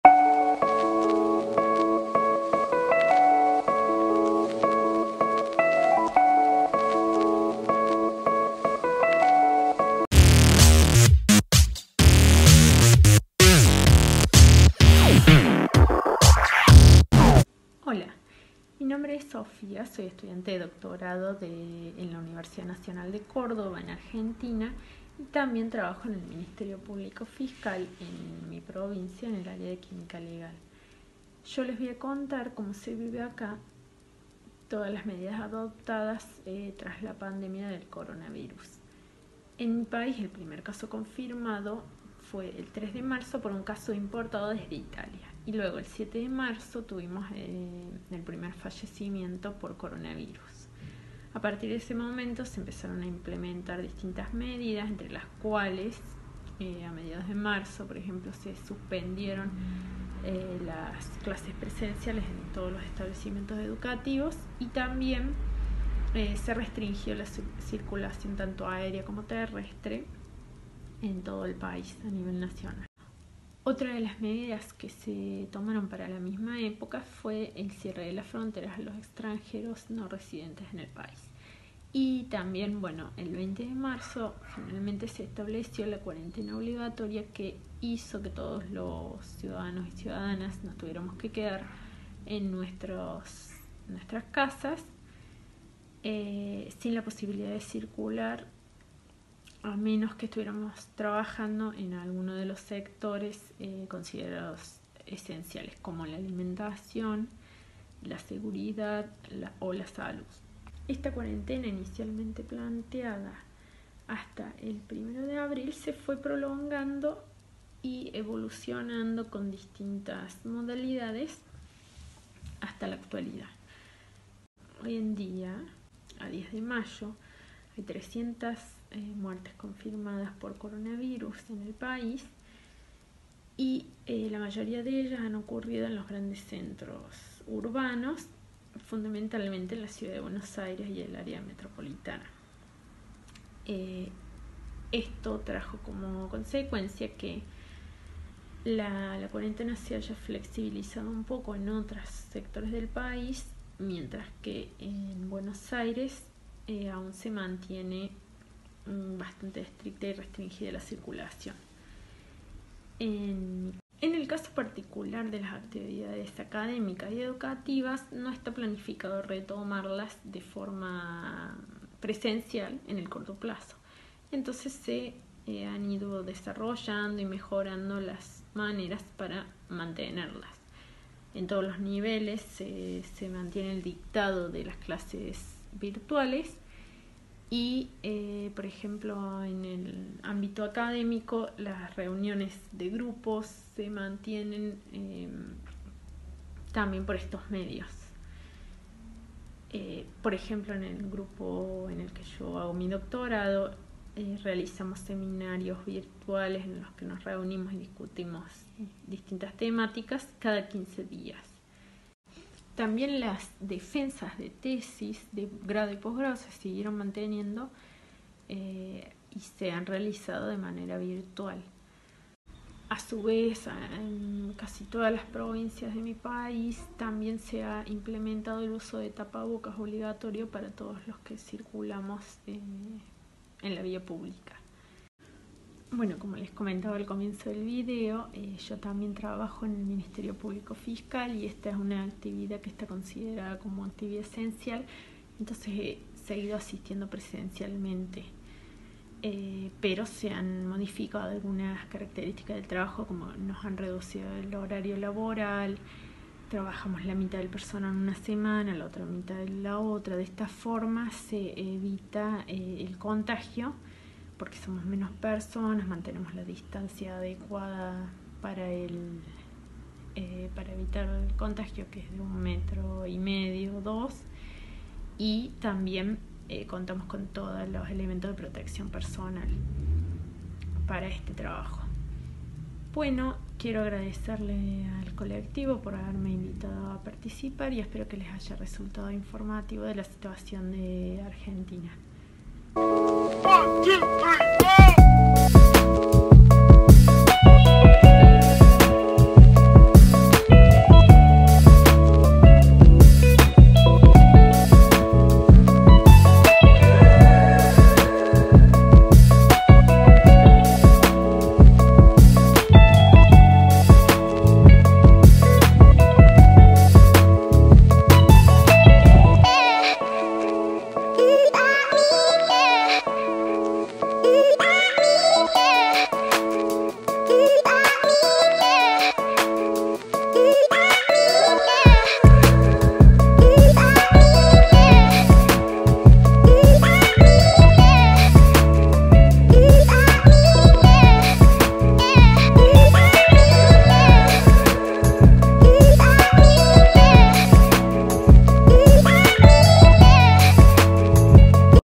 El Coronavirus en el Mundo: Argentina
Este es el noveno video de la serie “ElCoronavirus en el Mundo” esta vez toca viajar al continente Americano y más específico a la bella Argentina. En este nuevo capítulo acompáñanos a escuchar a nuestra querida amiga la Dra. Sofi Pestoni, ella nos hablará de la situación del Coronavirus en aquel país y las medidas que se han tomado.